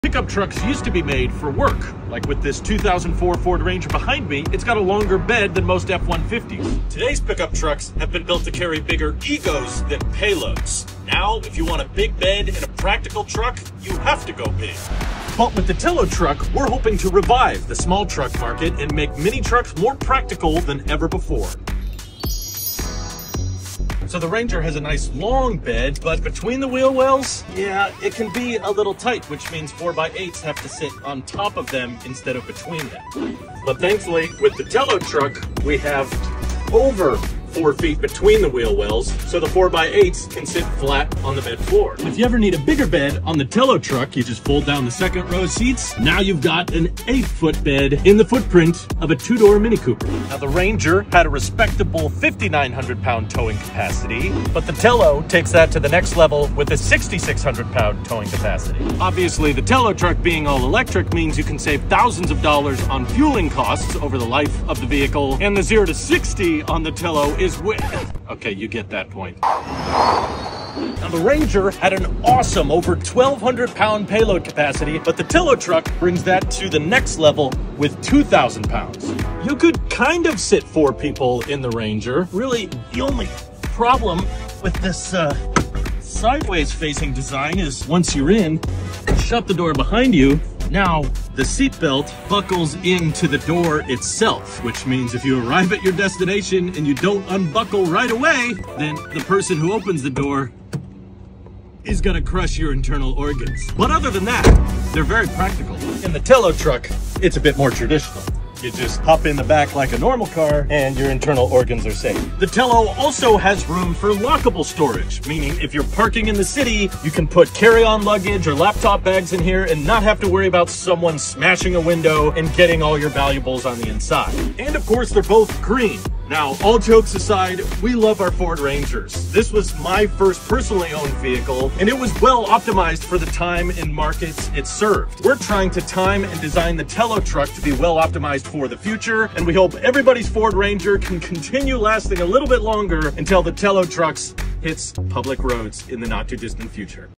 Ranger Inspired
TELO Trucks makes the world’s most efficient pickup with a 5 foot bed, 5 seats, and midgate that unlocks an 8 foot bed! TELO sports 350 miles of range and 500HP via a dual motor AWD powertrain while fitting in the footprint of a 2 door mini cooper!